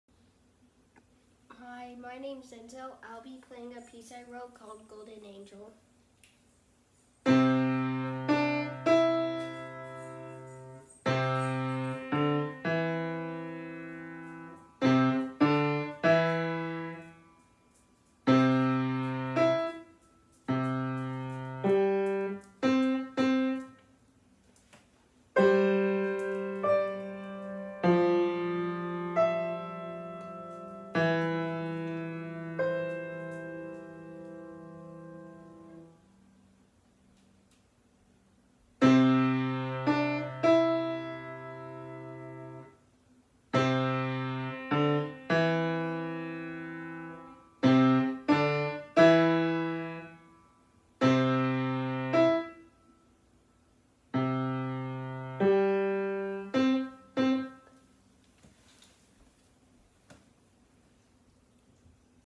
Enzo Lencioni Composed His Own Piece- Through Online Piano Lessons
Wow - Enzo composed his own music through online lessons!  He entered this video in the KMTA Summerfest.  Way to go, Enzo!